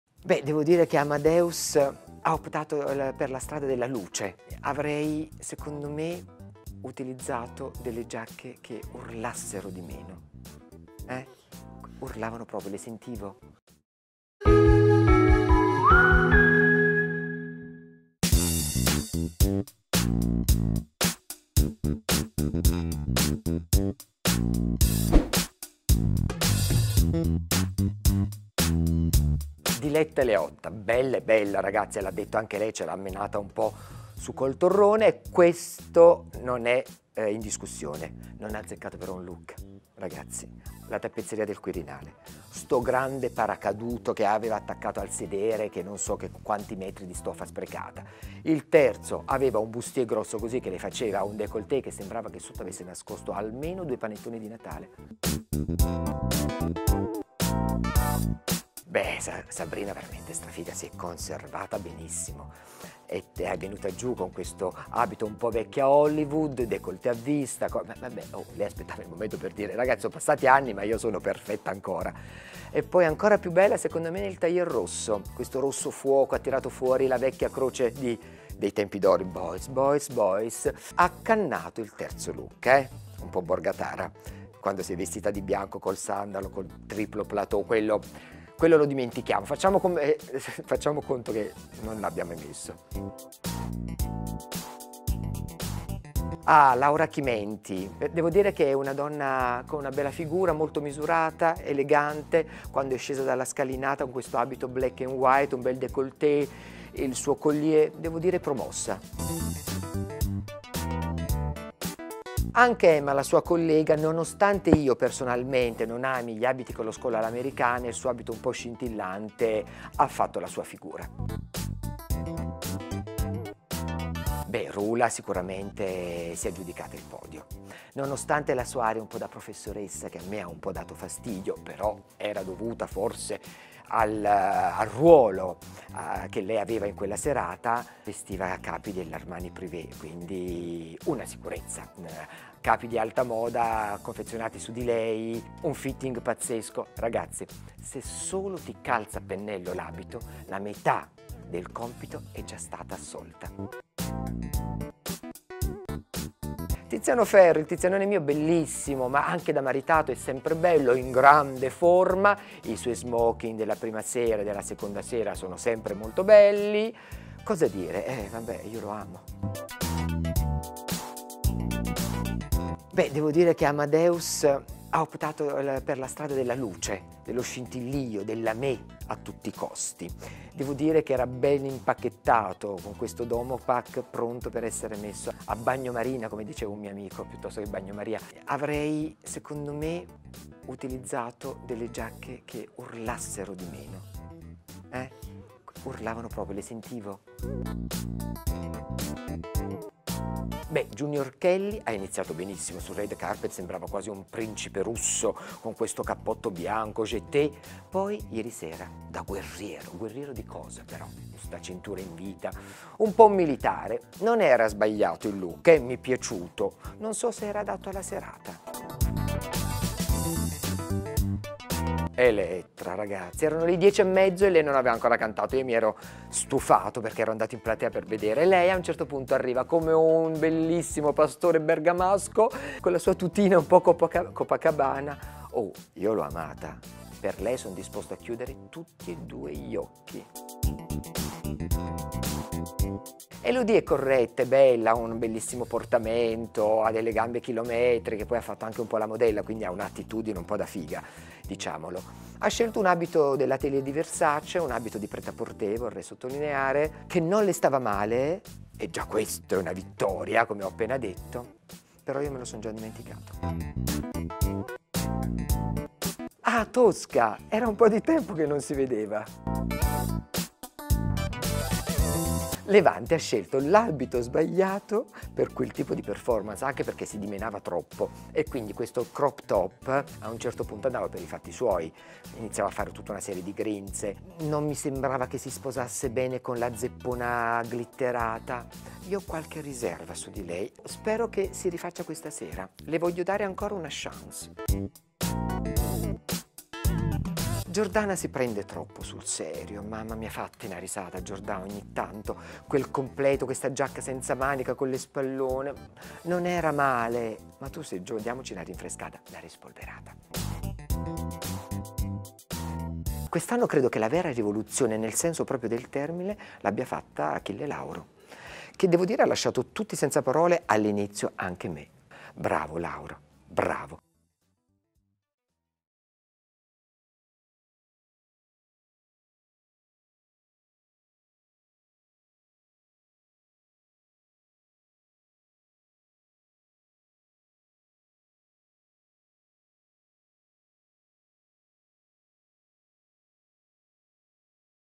Enzo Miccio a Sanremo 2020: le pagelle di stile
Come ogni anno, abbiamo intervistato Enzo Miccio durante il Festival di Sanremo, facendogli dare un voto allo stile di tutti coloro che hanno calcato il palcoscenico dell'Ariston. Le pagelle dei look dell'esperto di stile sono molto chiare: ha bocciato Diletta Leotto e Amadeus, mentre ha promosso Elettra Lamborghini e Achille Lauro.